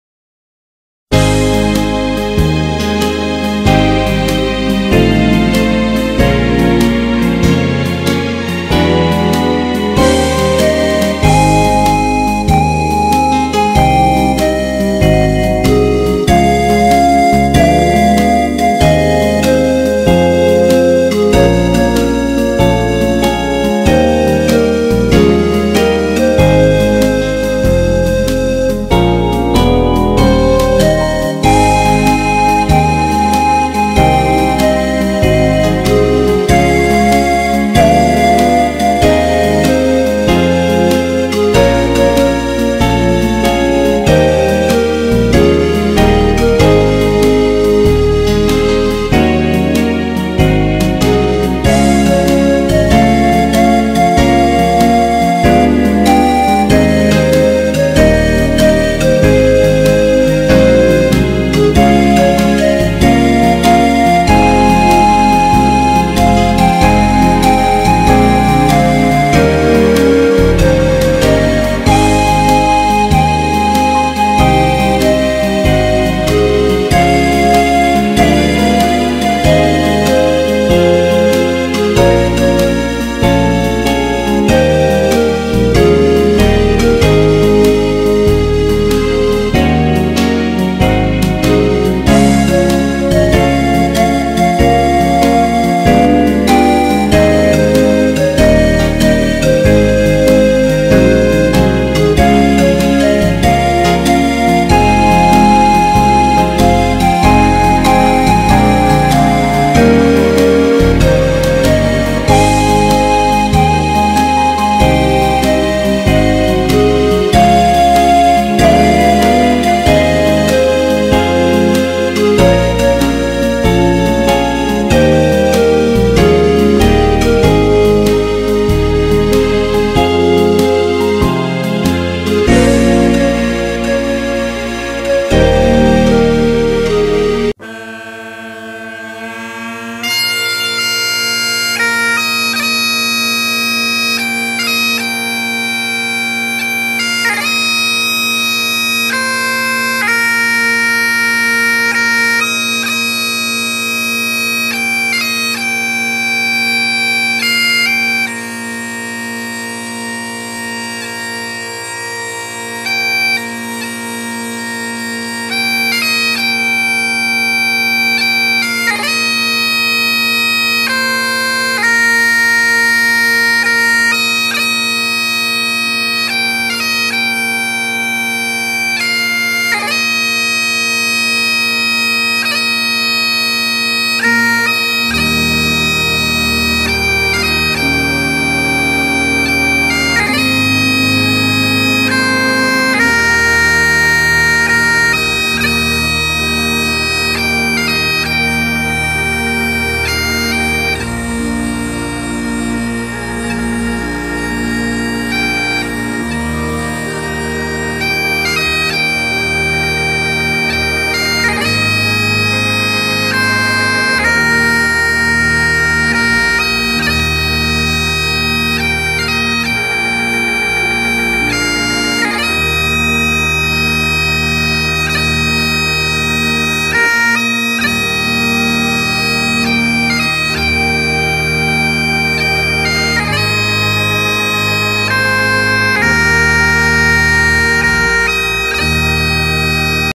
Victoria Cross.. S.Africa
Funeral of Captain Quinton Smythe,Natal Carabineers...the last surviving South African to hold the Victoria Cross